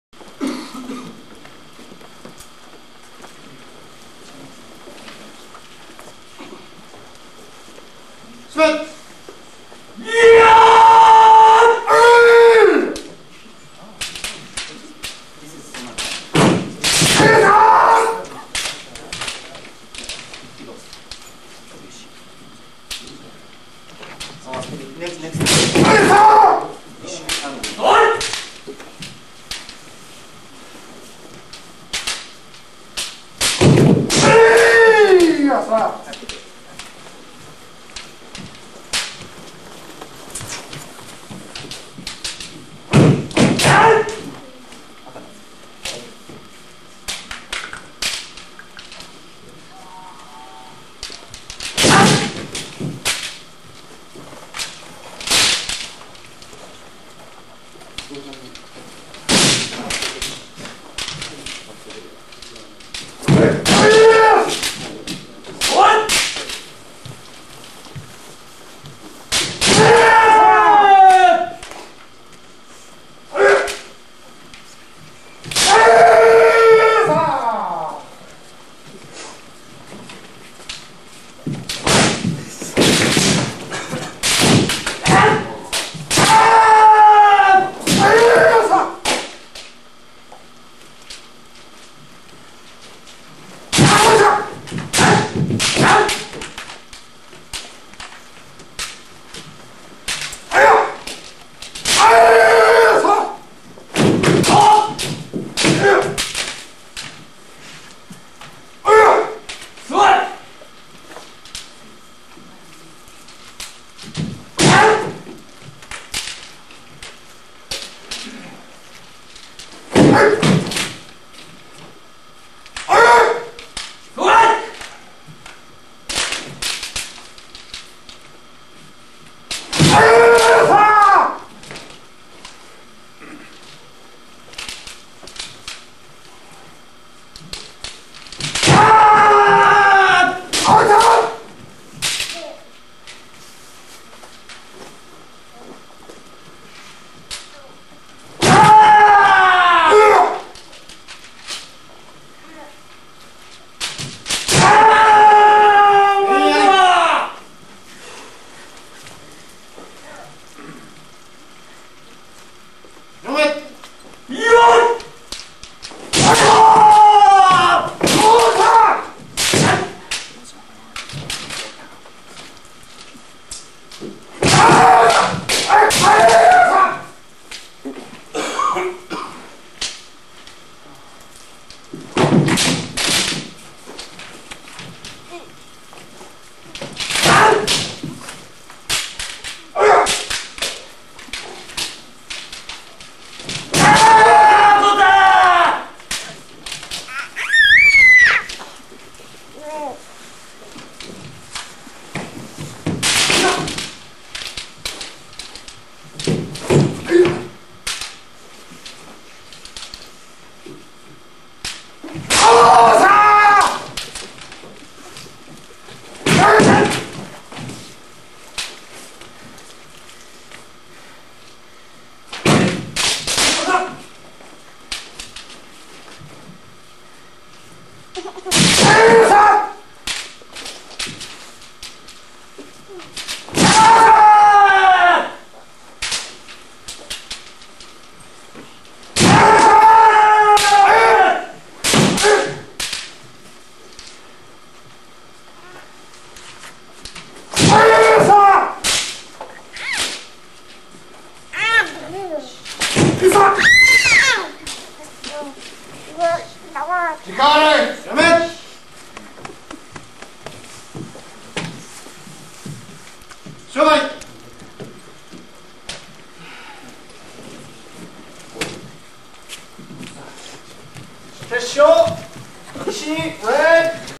Ito Vs Ruggiero. 2006 Monroe Kendo
Shunhei Ito of Miami Valley Vs Chris Ruggiero of Miami Valley at the 2006 Johnson Cup Kendo tournament in Monroe OH. 1-3 Dan division.